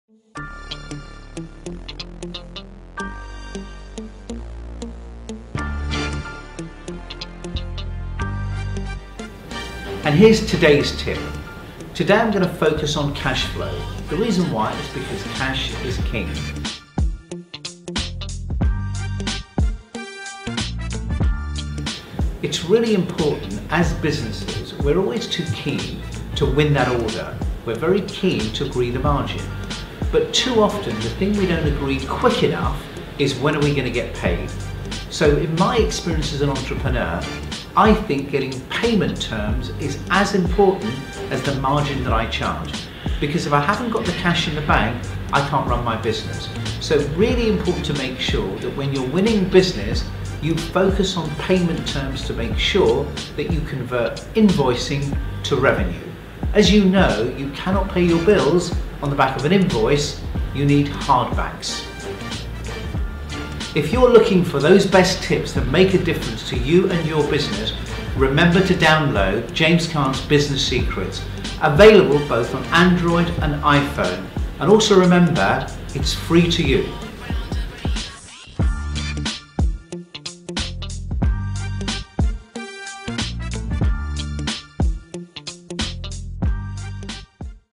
James Caan's Top Tips: Cash Is King
To celebrate reaching 1 million LinkedIn followers, James Caan is giving away tips via video all this week. Today he looks at an issue which often determines the success of a business - how much cash it has in the bank.

To follow James and read his highly acclaimed LinkedIn posts,